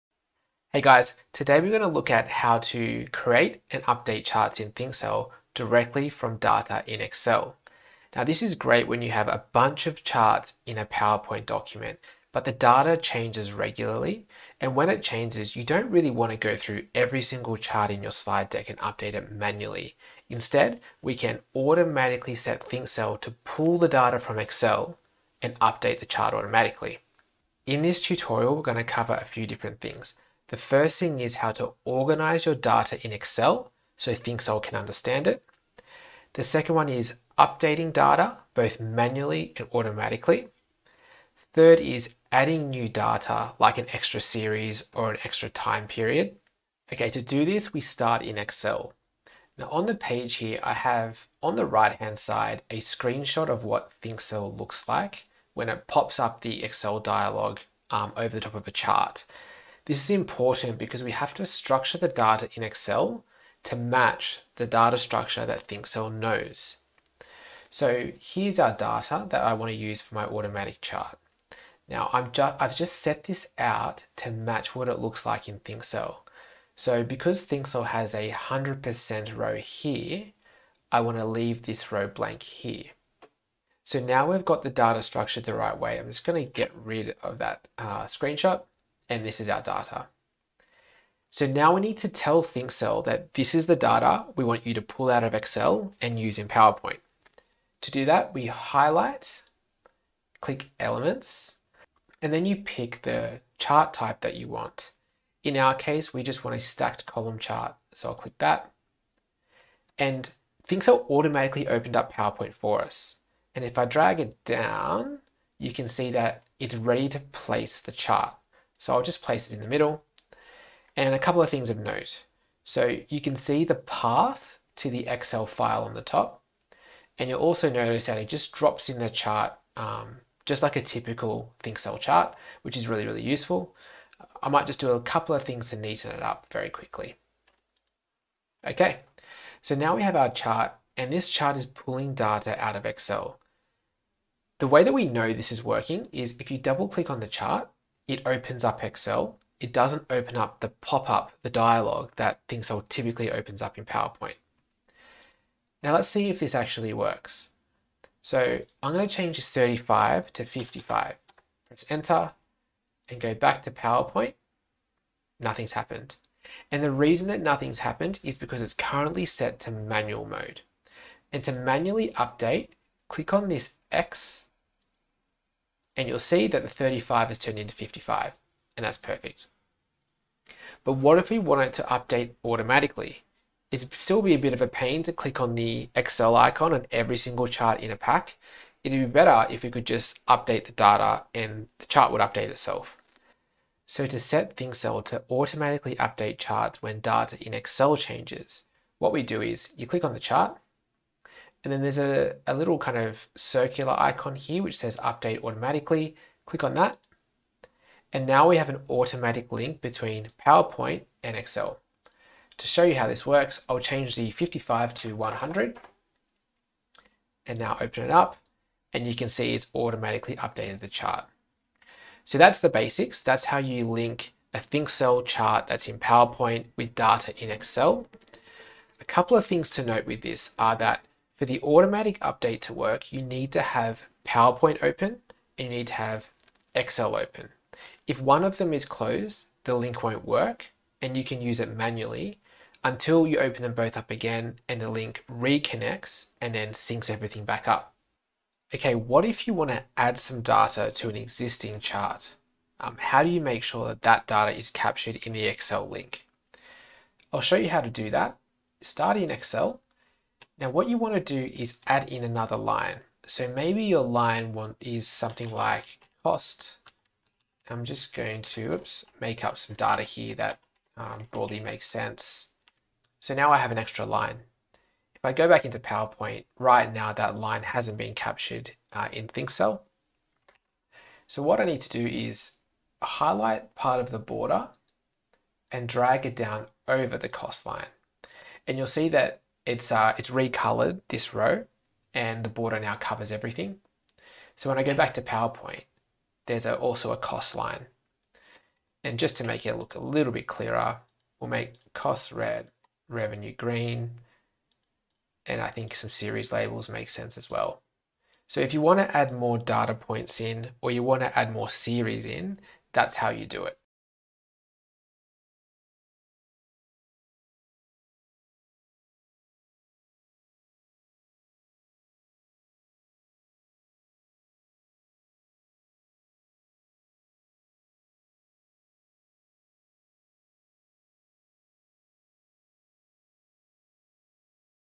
Update a Think-Cell Chart Dynamically from Excel Data
🎇 The Slide Science System (online course, slide templates, eBook, cheatsheets and more)
🏞️ The Business Strategy & Consulting Template Pack (176+ slides, 527+ shapes, 952+ icons)

In this video, we learn how to connect Think-Cell, PowerPoint and Excel, so that your chart automatically updates when data in Excel updates.

To do this:
1. Set up your data in Excel, so that it's in the same format as Think-Cell data
2. Highlight all cells with the data, then click Insert, Elements, and select your chart
3. This will automatically open up PowerPoint, so now place the chart.
4. Set chart to update automatically, by clicking on the chart, then clicking the circular 'Update Automatically' icon

Please note that both PowerPoint and Excel will be open for the data to update automatically. If either program is closed, the data can be edited manually, and will automatically update when both programs are open.

Chapters:
0:00 Introduction
0:46 Structuring your data in Excel
2:35 Updating data
4:08 Adding new data